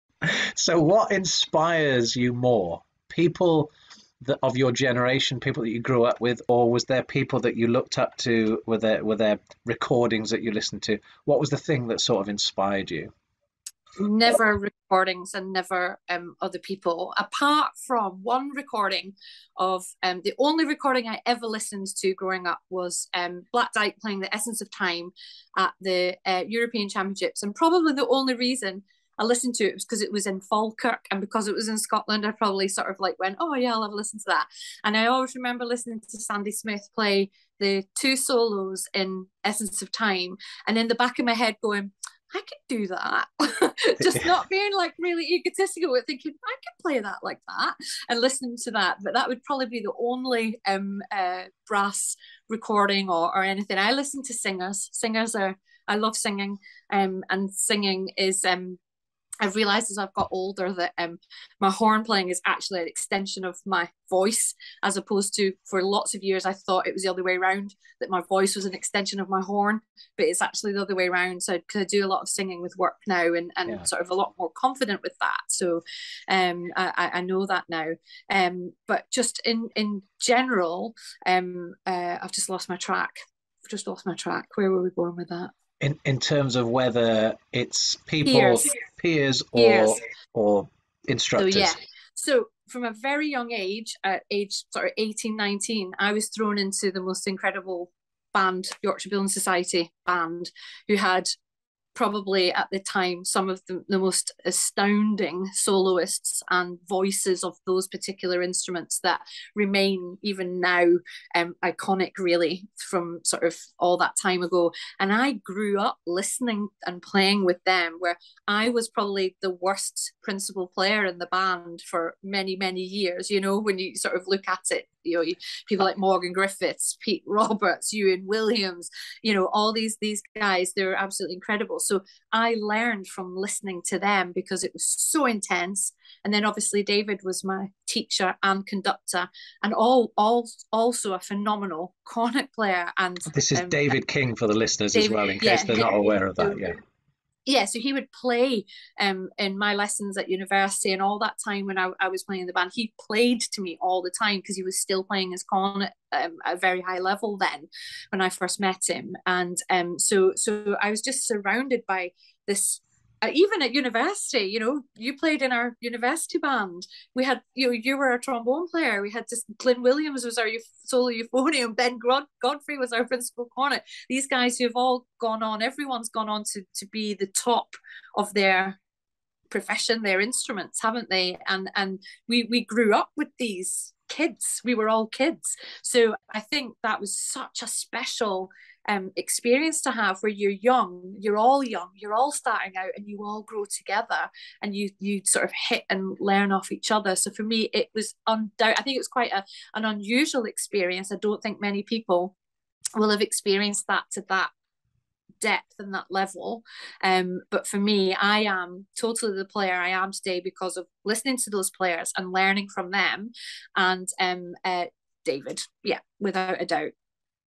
Were your peers or pros more influential? A T2P interview with Sheona Wade
Who was most influential to Denis Wick tenor horn artist Sheona Wade over the course of her musical career: her Peers or Pros? Find out in this transform2perform interview with Sheona Wade.

For more inspirational and educational Transform 2 Perform Denis Wick interviews, visit the Denis Wick App